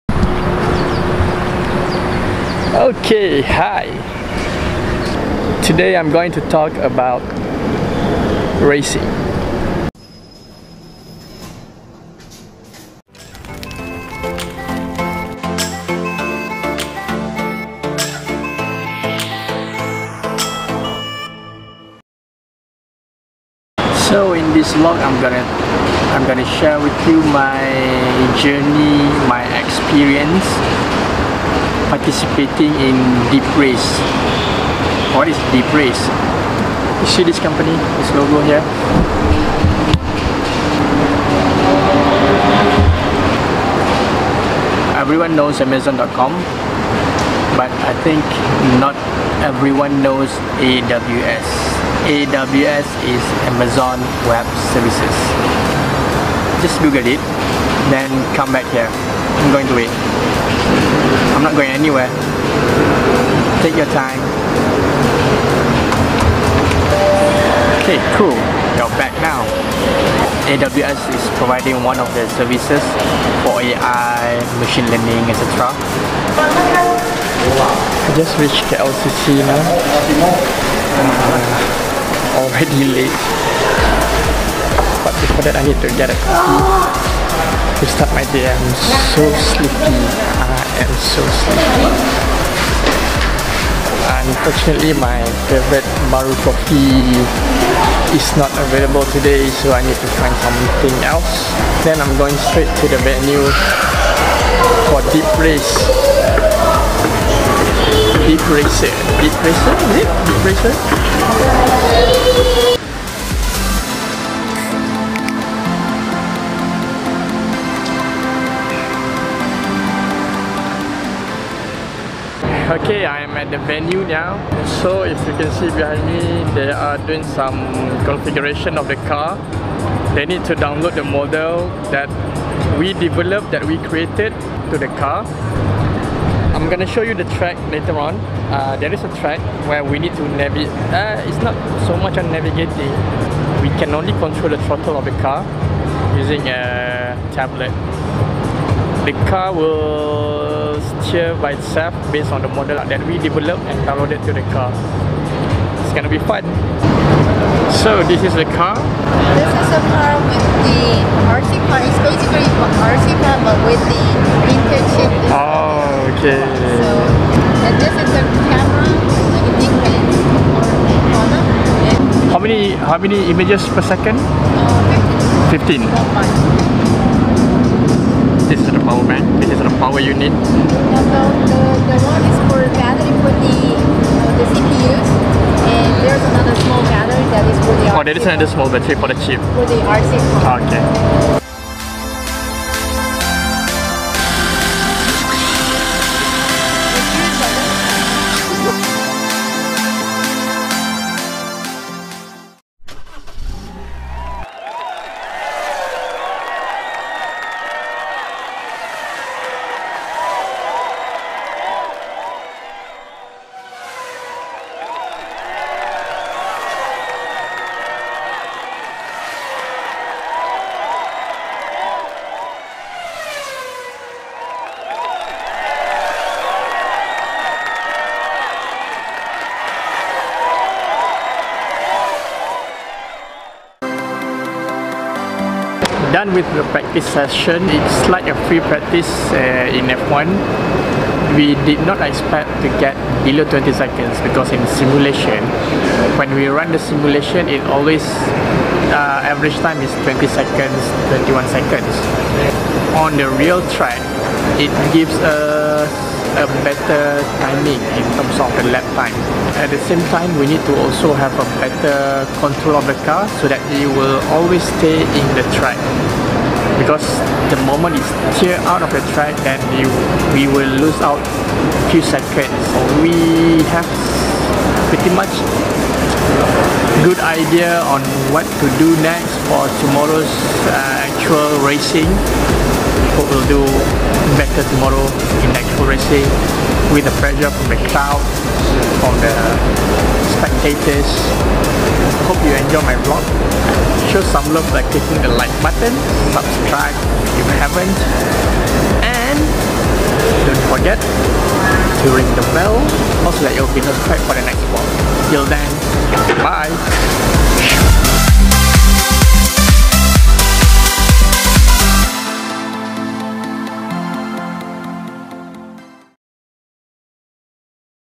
vlog #17 - I am a RACER!!!
DeepRacer by Amazon Web Services (AWS) is a fun way to learn about machine learning.

You don't need to master the programming language as the tutorial provided is good enough for you to start racing.

My Primary Camera
My Secondary Camera
The Mic That I Use
The Memory Card
The Editing Software: PowerDirector Mobile